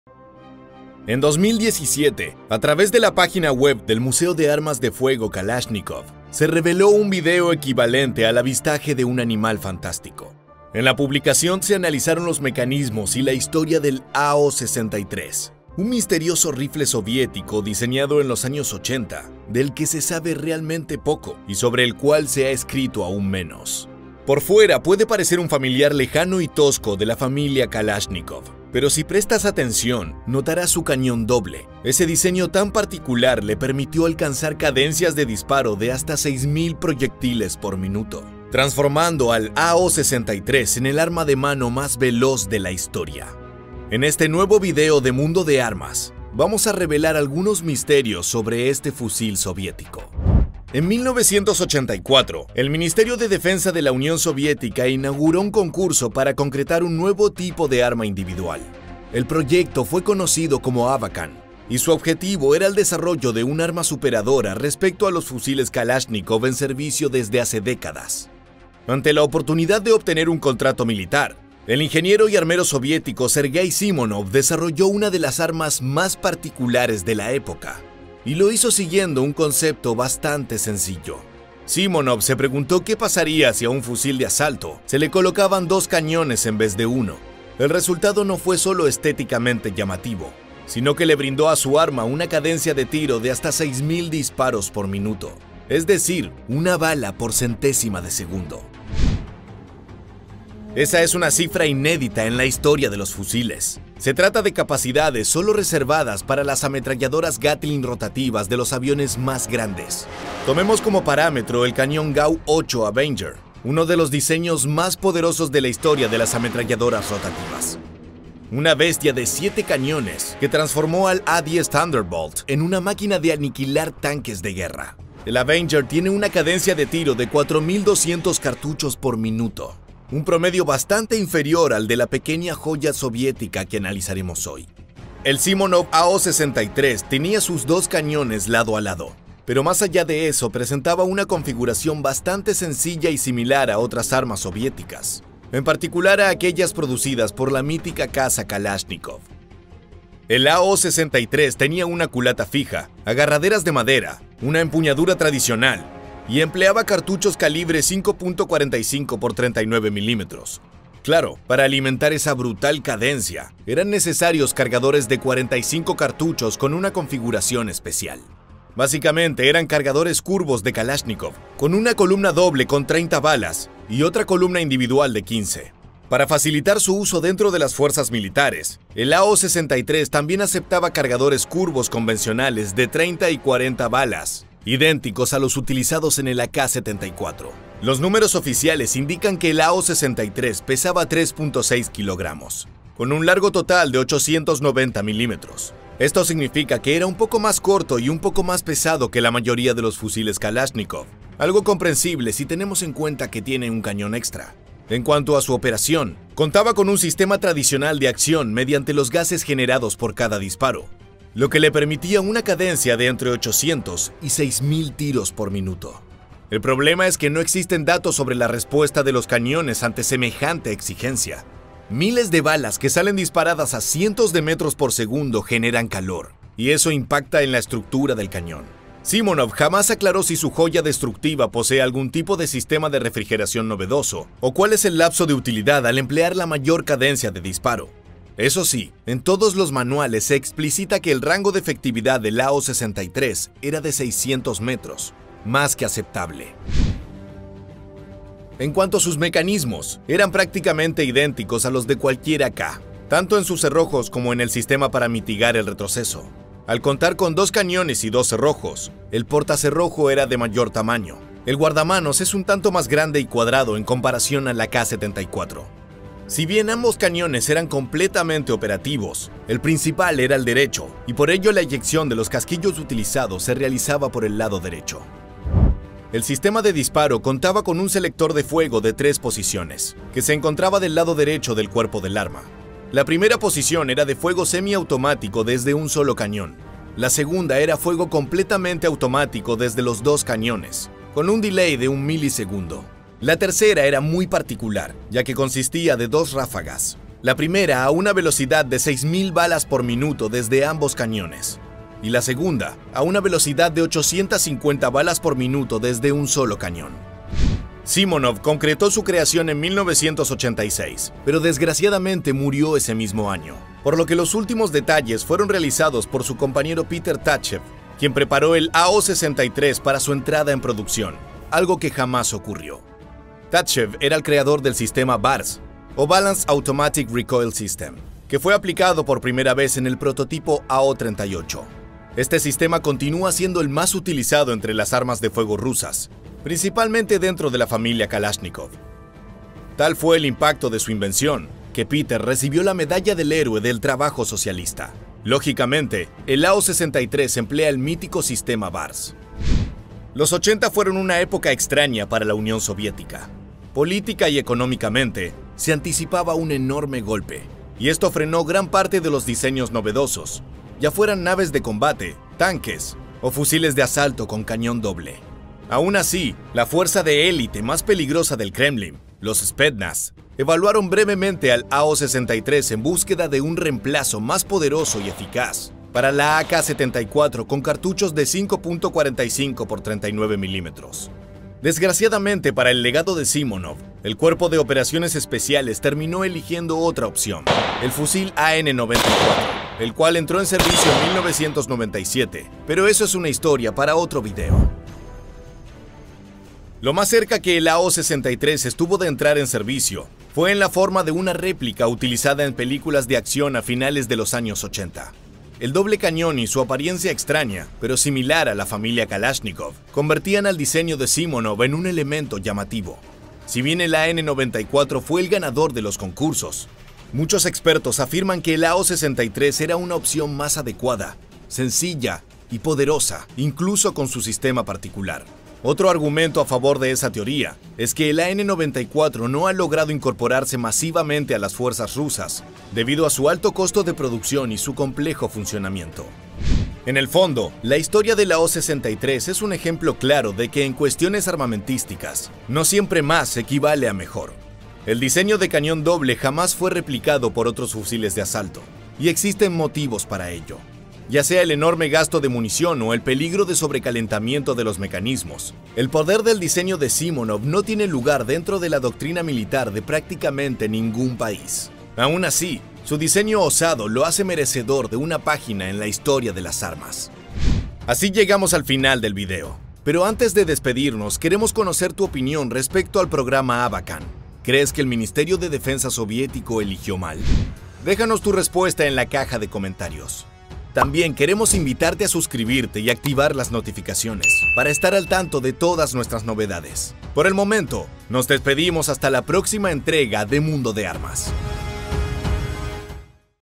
El rifle con la MAYOR cadencia de tiro… ¡100 balas en 1 segundo! | AO-63
¿Cómo es el AO-63? ¡Si quieres descubrir todos los detalles acerca del rifle con mayor cadencia de tiro no te pierdas este video!

En 2017, a través de la página web del museo de armas de fuego Kalashnikov, se reveló un video equivalente al avistaje de un animal fantástico. En la publicación se analizaron los mecanismos y la historia del AO-63, un misterioso rifle soviético diseñado en los años 80 del que se sabe realmente poco y sobre el cual se ha escrito aún menos.